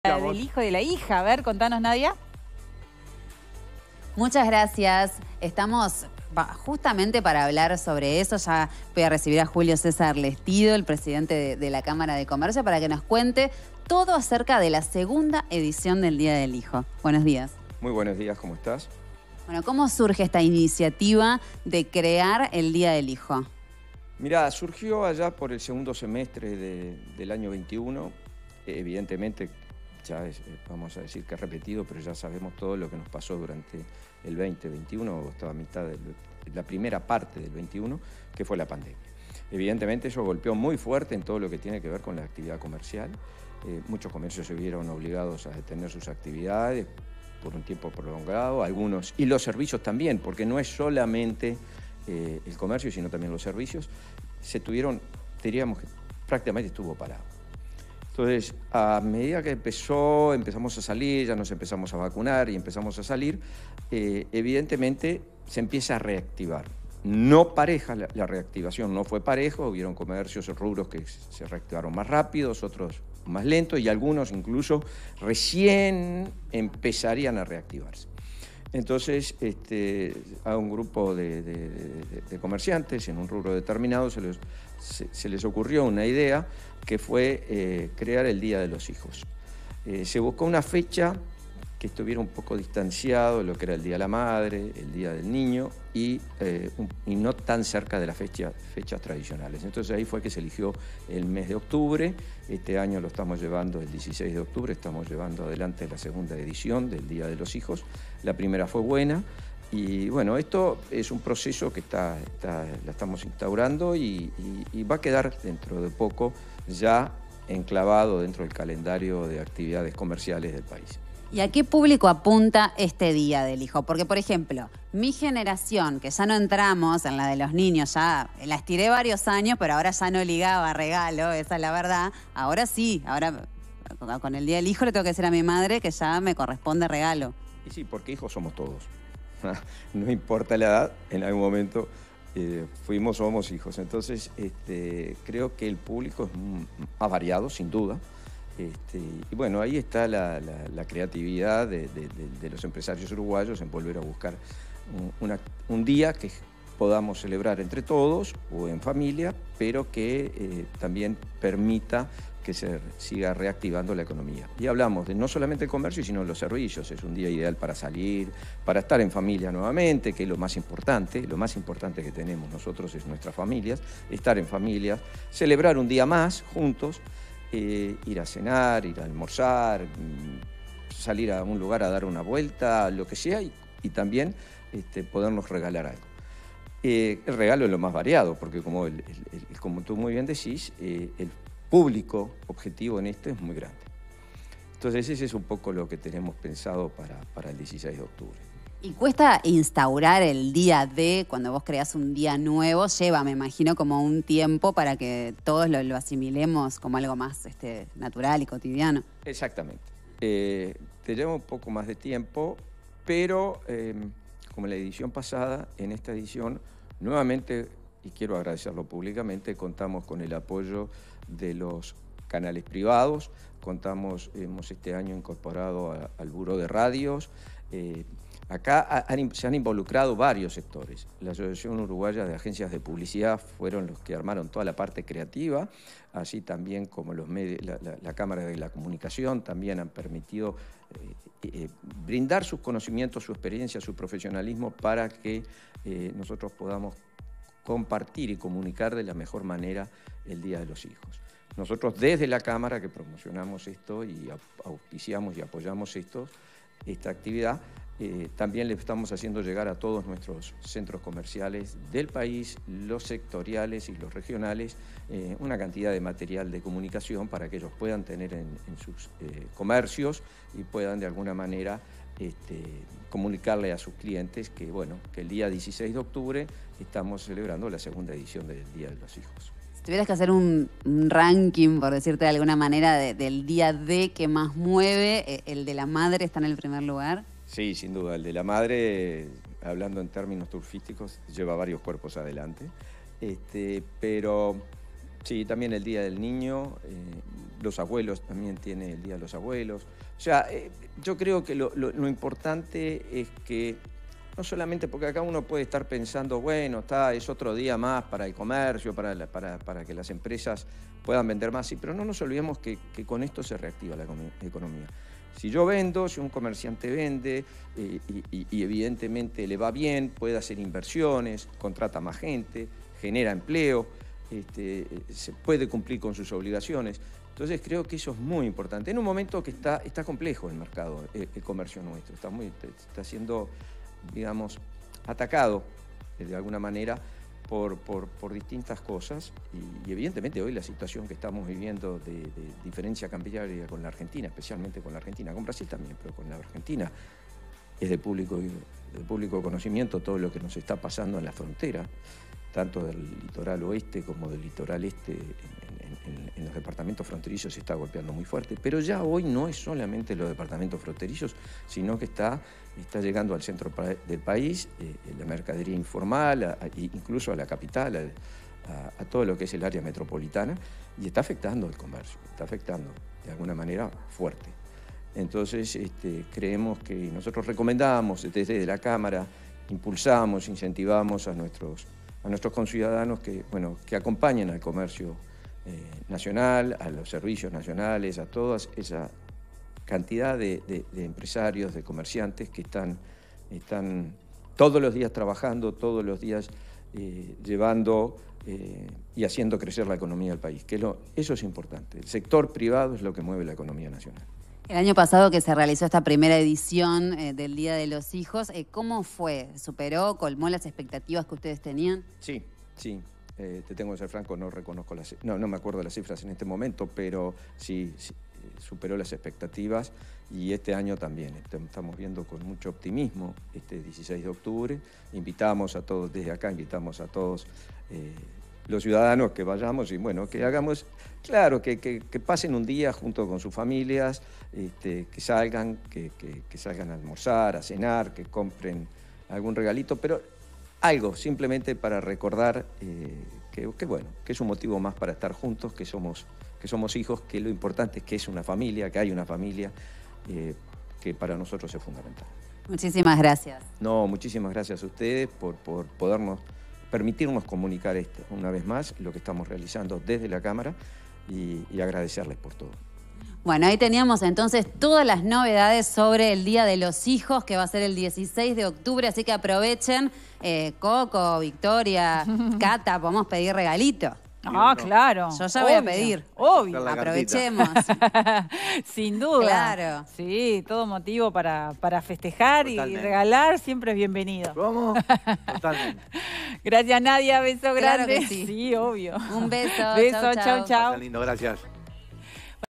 La Cámara de Comercio lanzó una nueva edición del Día de los Hijos
Nadia Fumeiro conversó con el presidente de la cámara, Julio César Lestido, quien nos contó todo sobre acerca del próximo 16 de octubre.